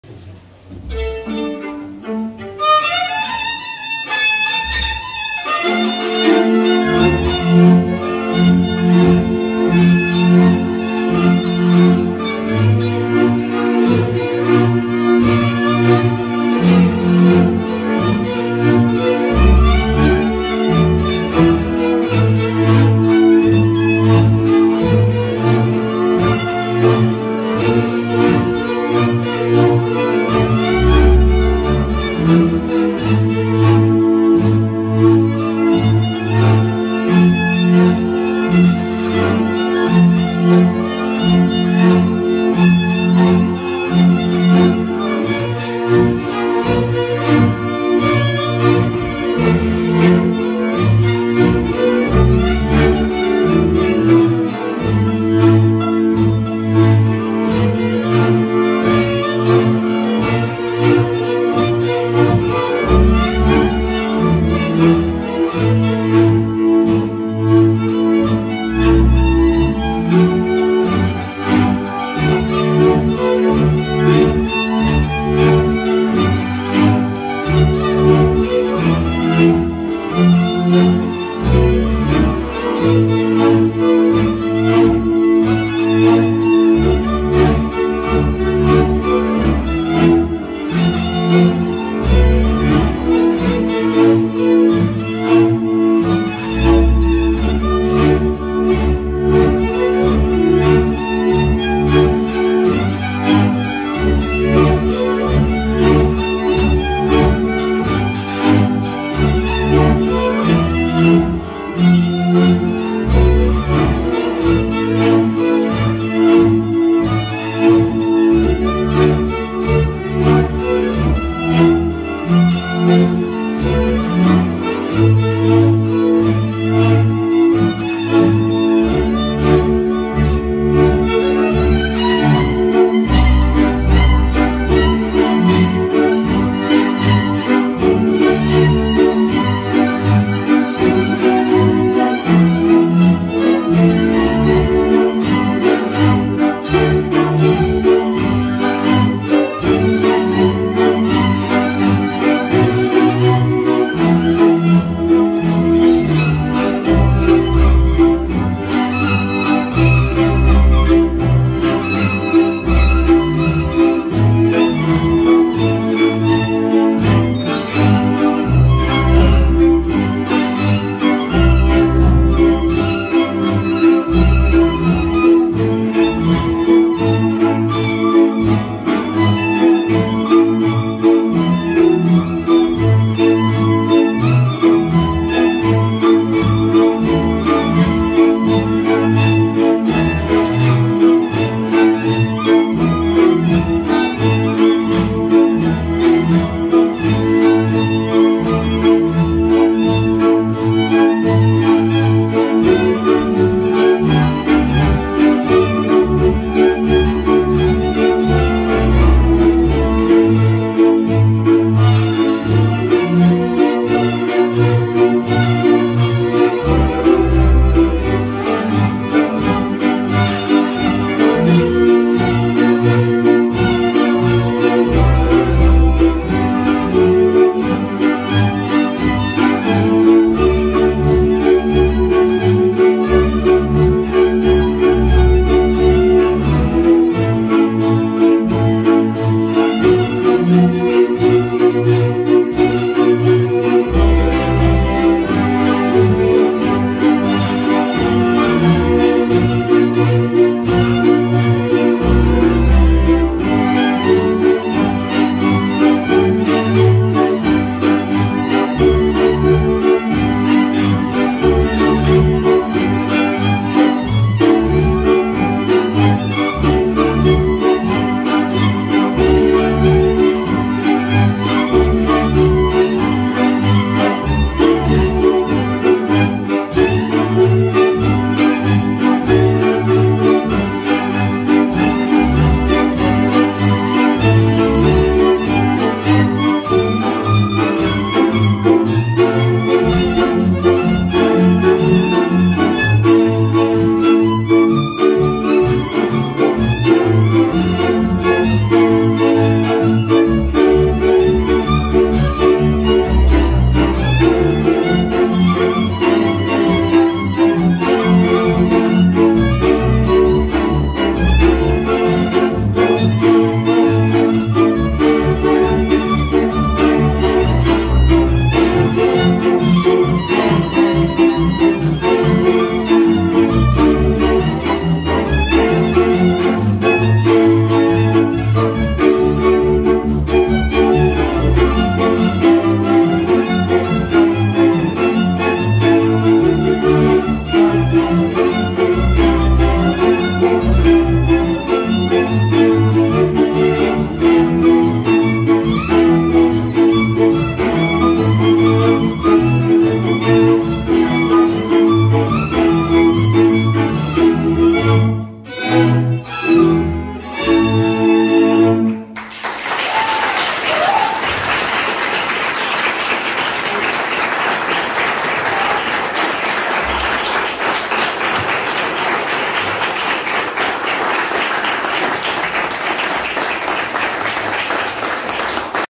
Codoba Florinel és bandája, Kallós Zoltán Népzeneiskola záró szám
@Heltai Folk Center, Kolozsvár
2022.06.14

Codoba Florinel
Codoba Marius
Szakács Kristóf
Mezei Karsa